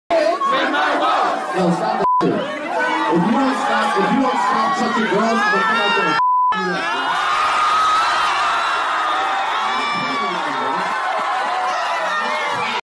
Drake calls out guy in crowd 'touching girls' during Sydney show.
Drake calls out guy 'touching girls' during Sydney show